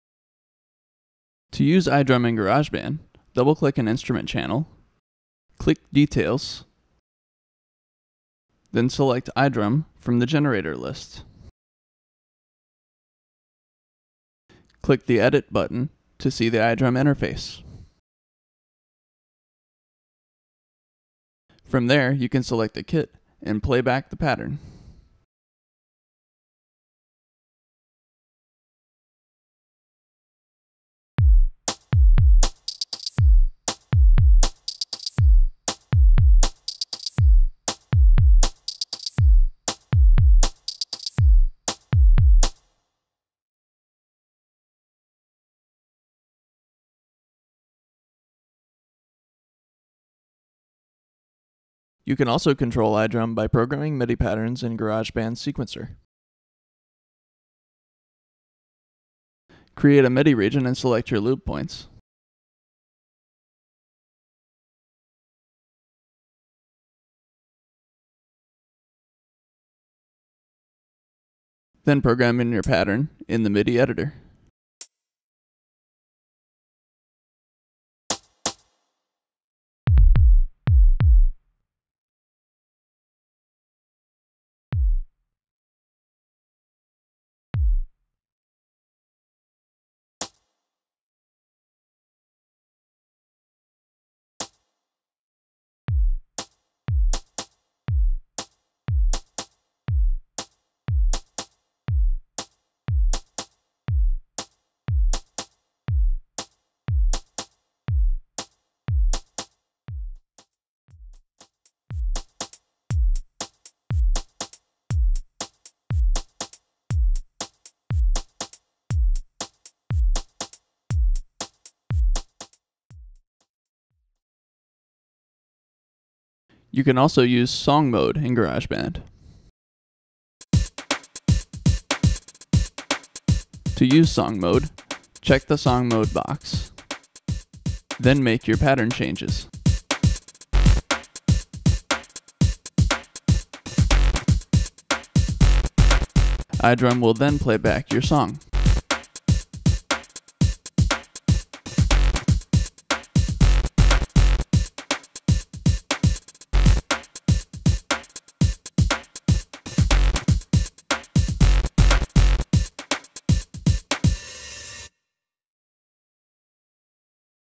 Izotope iDrum & Garageband
Using Izotope iDrum, the drum machine for Mac OSX with Garage Band.